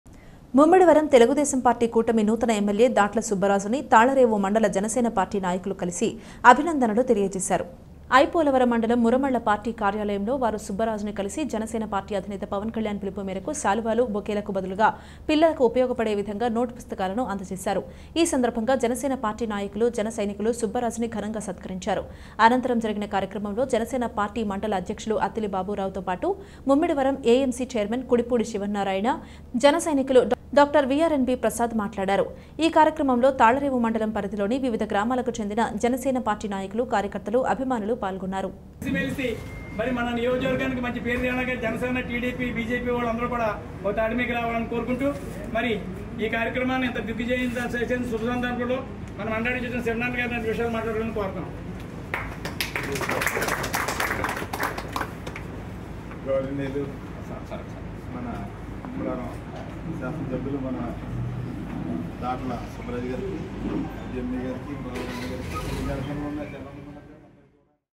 కూటమి నూతన ఎమ్మెల్యే దాట్ల సుబ్బరాజుని కలిసి అభినందనలు తెలిపిన తాళ్లరేవు మండల జనసేన పార్టీ నాయకులు
ముమ్మిడివరం తెలుగుదేశం పార్టీ కూటమి నూతన ఎమ్మెల్యే దాట్ల సుబ్బరాజుని తాళ్లరేవు మండల జనసేన పార్టీ నాయకులు కలిసి అభినందనలు తెలియజేశారు.